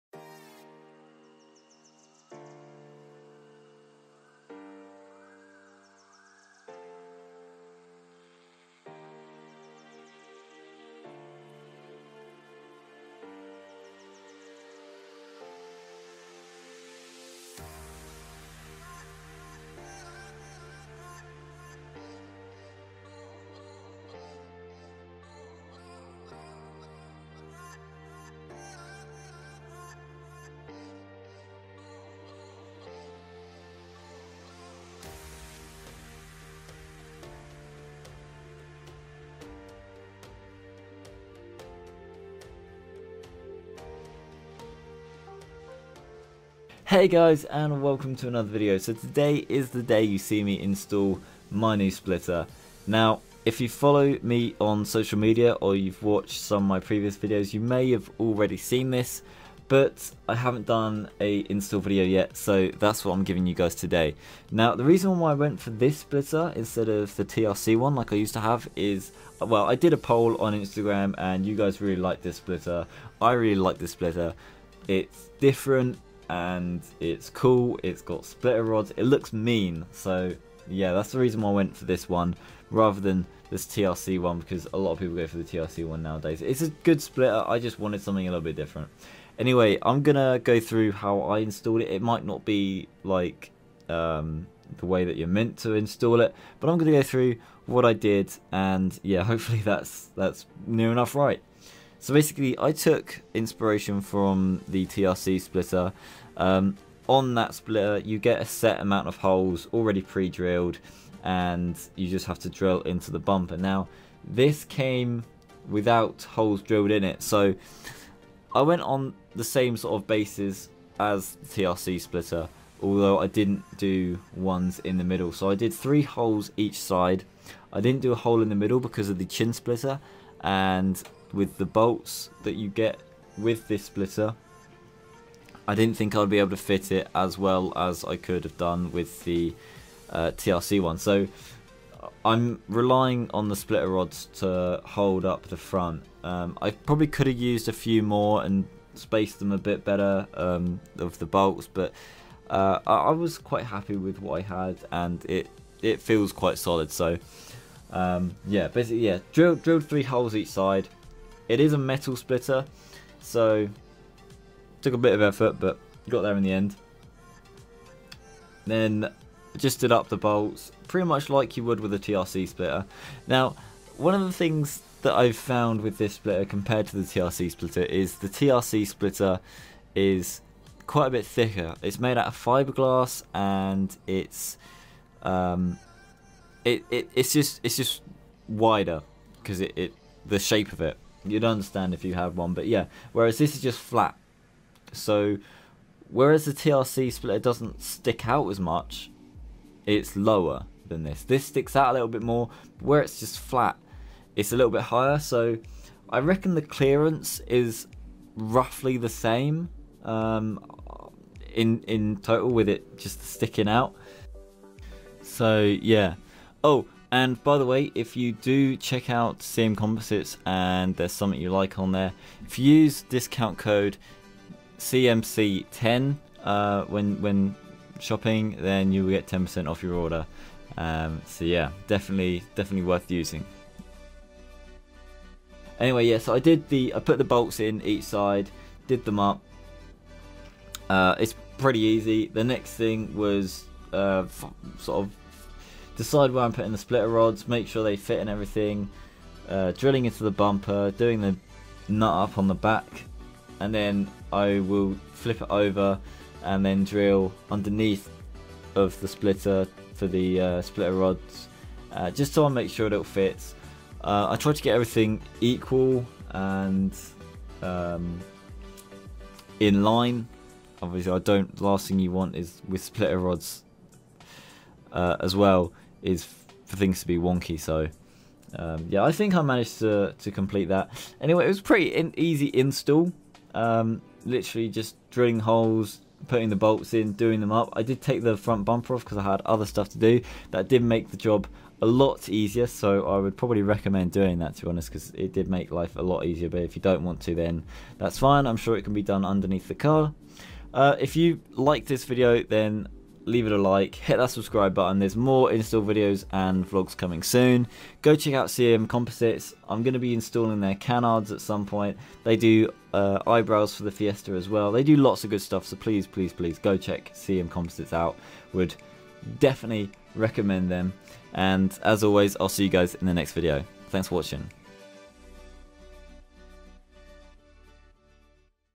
CMComposites Race Style Splitter Install (Ford Fiesta)
*I Fitted this splitter how I saw fit, others may install it differently*
So i got my car a new splitter. I swapped out my broken Triple R Composites one for a CMComposites Race Style Splitter. Check out their website for great products!
I would totally recommend this product, it looks absolutely fantastic! you may even see some CMComposites Side Skirts on my car in the near future!
This splitter fits the Fiesta ST MK7 and Fiesta MK7 PFL/ Fiesta MK 7.5 FL
I also have some Canards to fit so you will be seeing more of their products for sure!